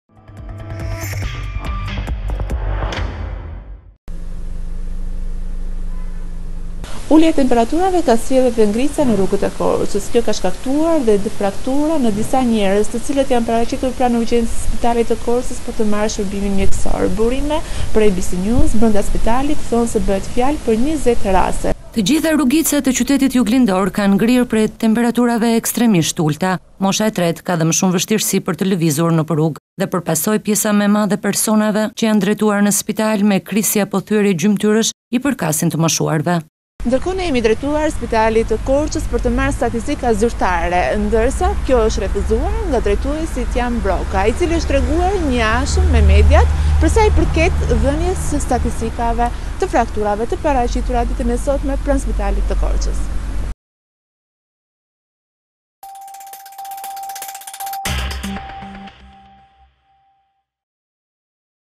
“Ngrin” Korca, 20 persona kerkojne ndihme mjekesore, me fraktura ne gjymtyre | ABC News Albania
Në Qarkun e Korçës ka ngrica për shkak të temperaturave të ulëta. Burime të “ABC News” pranë spitalit të Korçës bëjnë me dije se 20 persona kanë kërkuar ndihmë mjekësore për shkak të frakturave në gjymtyrë. Gazetarja Ledjona Xheladinaj kërkoi informacion zyrtar, por drejtoria e spitalit rajonal Korçë, refuzoi të përgjigjet.

Burime te “ABC News” raportojnë se 20 persona kanë shkuar në spital sepse kanë rrëshqitur dhe kanë pësuar fraktura.

Të gjitha rrugicat e qytetit juglindor kanë ngrirë prej temperaturave ekstremisht të ulëta. Mosha e tretë e ka edhe më shumë vështirë për të lëvizur nëpër rrugë dhe për pasojë pjesa më e madhe e personave që janë drejtur në siptal me krisje apo thyerje gjymtyrësh i përkasin të moshuarve.

“Abc News” kërkoi informacion zyrtar në spital, por drejtori refuzoi kërkesën./abcnews.al